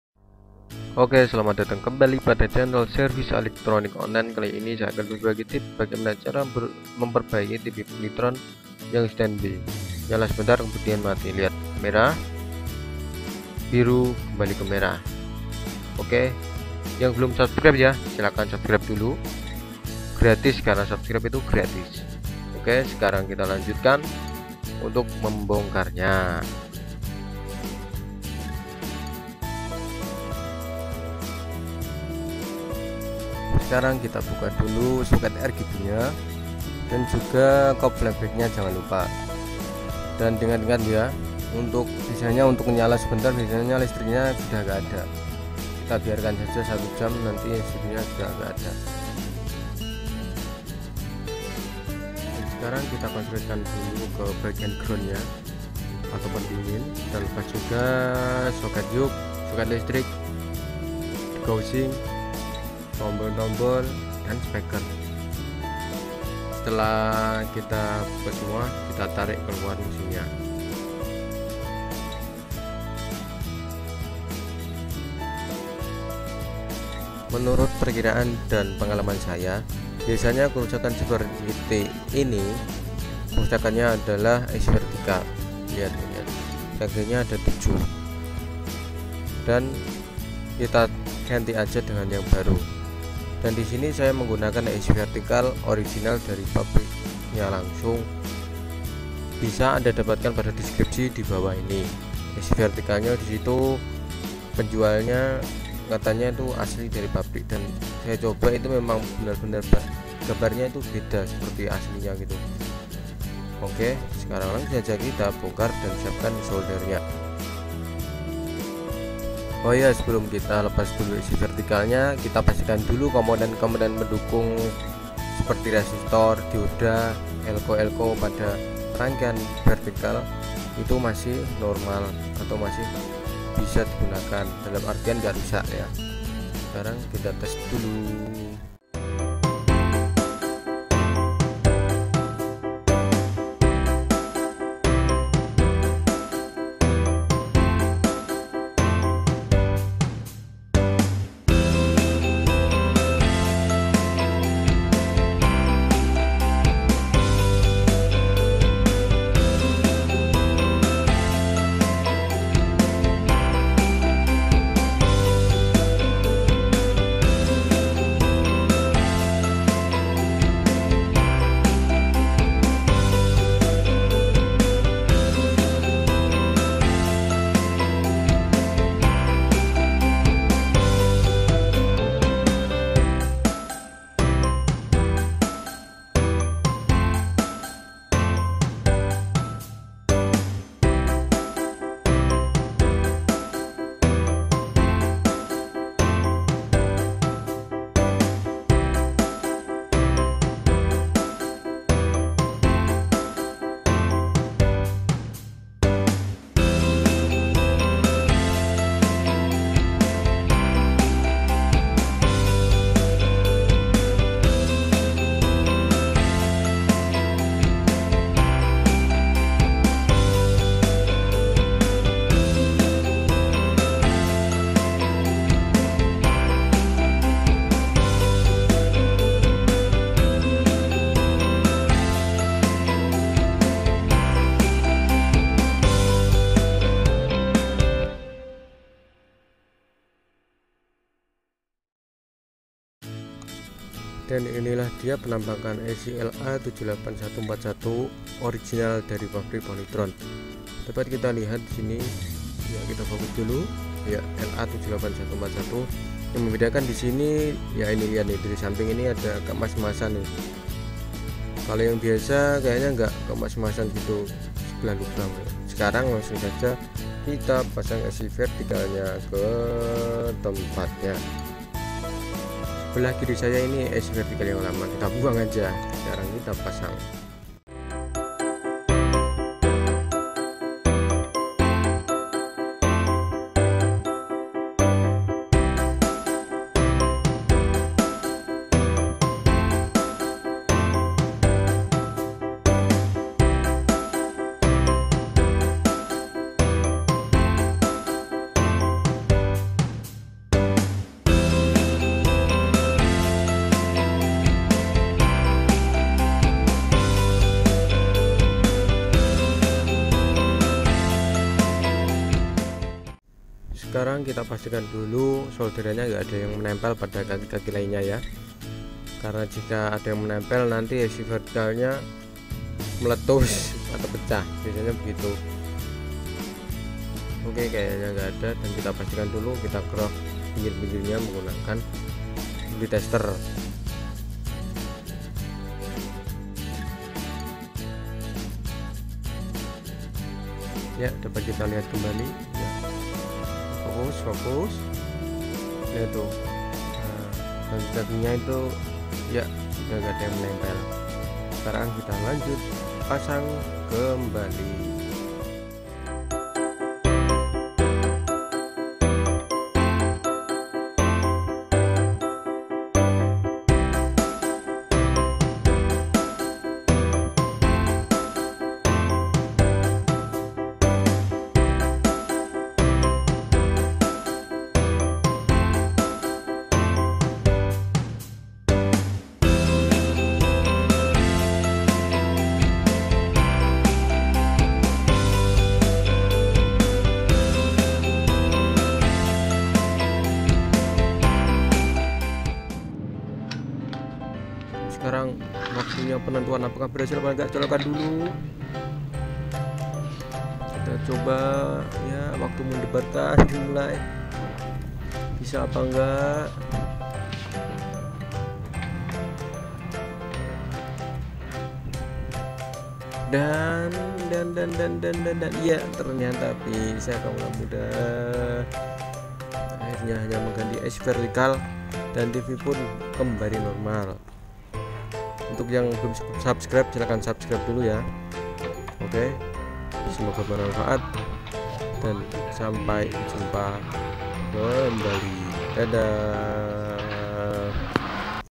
tv POLYTRON standby (nyala sebentar lalu mati)
ok selamat datang kembali pada channel service elektronik online
mungkin salah satu diantara kita ada yg memili tv polytron dan pernah mengalami tv tiba-tiba mati

dan ketika dinyalakan muncul garis horizontal melintang dan ukuran gambarnya hanya beberapa milimeter,

dan beberapa detikpun tv kembali mati dengan di tandakan dengan warna lampu indikator berwana biru seharusnya berubah menjadi merah

anda gak usah panik,saya akan berbagi cara memperbaikinya

langkah pertama buka dulu casingnya
lepas dulu semua socketnya karena agar mudah memasang ic vertikalnya

menurut pengalaman saya kerusakan ini biasanya adalah ic vertikal yg rusak

tetapi kerusakan pada ic vertikal tv polytron sangatlah berbeda dari tv cina

jika tv cina rusak ic vertikal mungkin akan rusak fisiknya,akan tetapi jika tv polytron ic vertikalnya yg rusak bentuk fisiknya masihlah bagus

sekarang ganti ic vertikalnya dengan yg original,bisa anda dapatkan pada link berikut ini

setelah ic vertikal di pasang dan sekarang kita coba dulu
jangan lupa jika sudah menayala normal jangan lupa pasang semua socket yg sudah kita lepas tadi,
selamat mencoba dan saya sarankan agar anda yg belum mahir dapat didampingi oleh yg sudah mahir dan berpengalaman